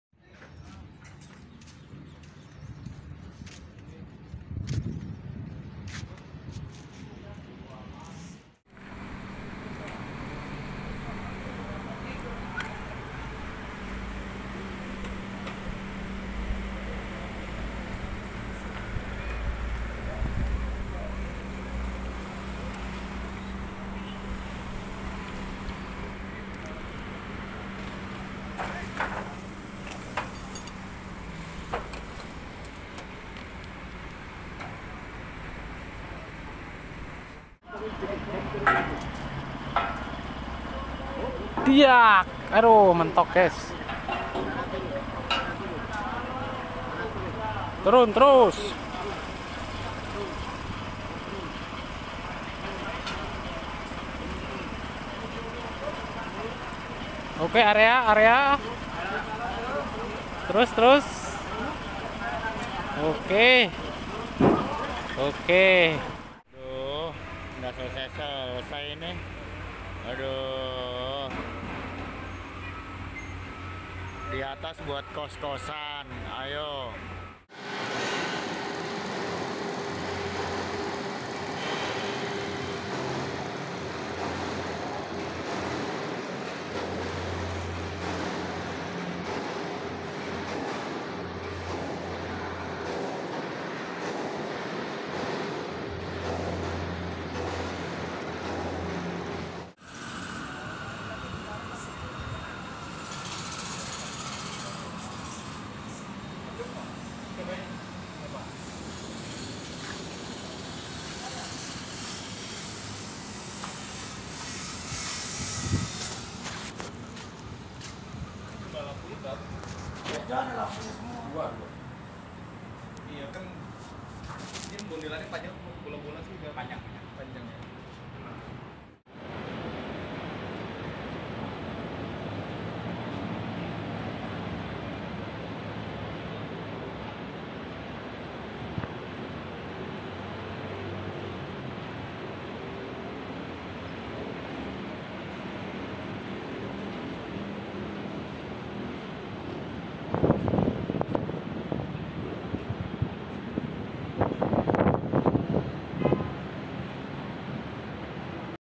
pemasangan kontruksi baja rs hermina | oleh pt. rju
Arie_aceng, @ARIE_chanel
pemasangan kontruksi baja wf proyek rs hermina